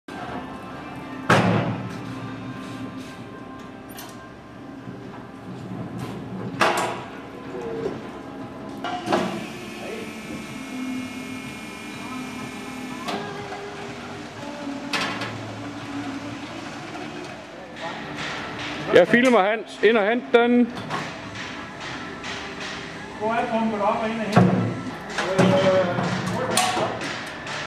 Easy in-feed and removal of pallets from a conveyor system
By installing a low-built lifting table it is possible to feed pallets into a conveyor system using a manual pump truck.